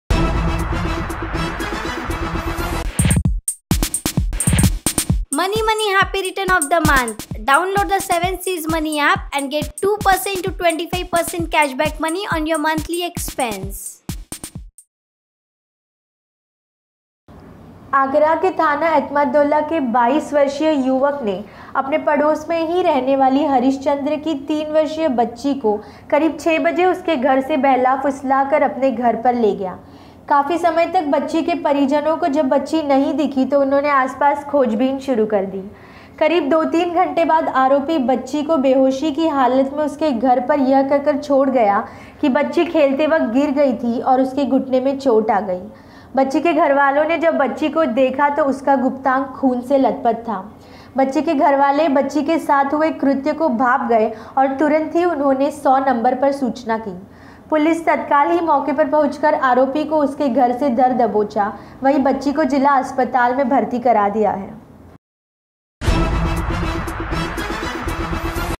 पड़ोसी ने ही किया शर्मनाक कृत्य
3 साल की बच्ची के रेप का मामला